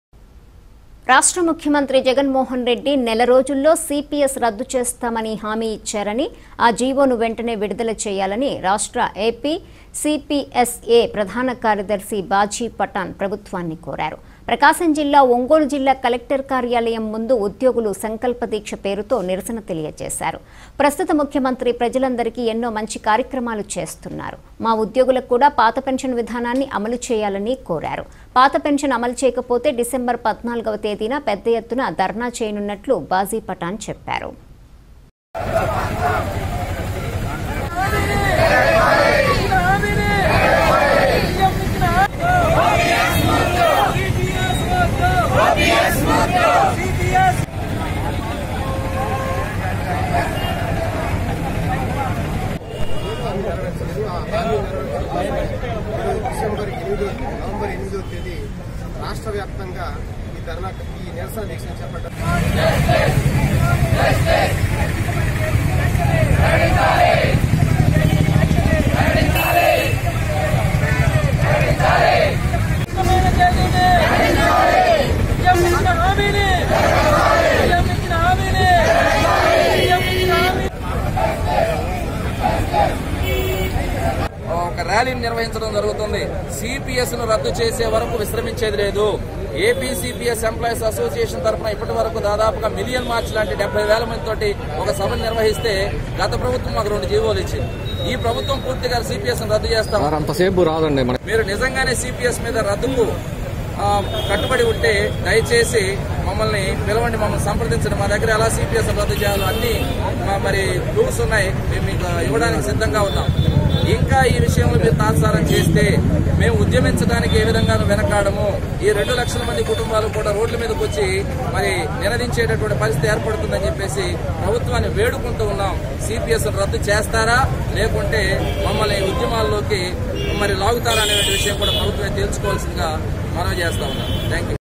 ప్రకాశం జిల్లా ఒంగోలు జిల్లా కలెక్టర్ కార్యాలయం ముందు ఉద్యోగులు సంకల్ప దీక్షపేరుతో నిరసన తెలియజేశారు
.రాష్ట్ర ముఖ్యమంత్రి జగన్మోహన్ రెడ్డి నెలరోజుల్లో సిపిఎస్ రద్దు చేస్తామని హామీ ఇచ్చారని ఆ జీవోను వెంటనే  విడుదల చేయాలని  రాష్ట్ర ఏపీ సిపిఎస్ ఏ ప్రధాన కార్యదర్శి బాజీ పటాన్ ప్రభుత్వాన్ని కోరారు. ప్రకాశం జిల్లా ఒంగోలు జిల్లా కలెక్టర్ కార్యాలయం కార్యాలయం ముందు ఉద్యోగులు సంకల్ప దీక్ష పేరుతో నిరసన తెలియజేశారు. ప్రస్తుత ముఖ్యమంత్రి ప్రజలందరికీ ఎన్నో మంచి కార్యక్రమాలు చేస్తున్నారు మా ఉద్యోగులకు కూడా పాత పెన్షన్ విధానాన్ని అమలు చేయాలని కోరారు.  పాత పెన్షన్ అమలు చేయకపోతే డిసెంబర్ 14వ తేదీన పెద్ద ఎత్తున ధర్నా చేయనున్నట్లు బాజీ పటాన్ చెప్పారు. ఏపీ సి పి ఎస్ఇ ఏ రాష్ట్ర ప్రధాన కార్యదర్శి  బాజీ పటాన్ వాయిస్. ఏపీ సిపిఎస్ ఇ ఏ  జిల్లా అధ్యక్షులు ప్రసాద్ వాయిస్.